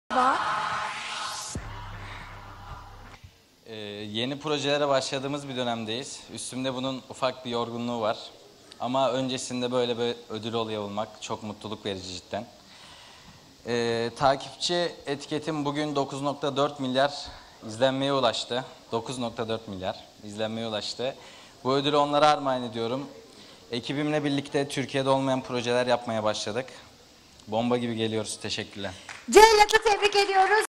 ''Ekibimle beraber Türkiye'de olmayan projeler yapmaya başladık.''
MüzikonairÖdülleri 2021 - Yılın Erkek Tiktokerı Cellat oluyor.

5. BAU Radyo Müzikonair Ödülleri 7 Şubat 2022 Pazartesi günü sahiplerini buldu. Çok sayıda ünlü ismin katıldığı tören Dream Türk TV ekranlarından canlı yayınlandı.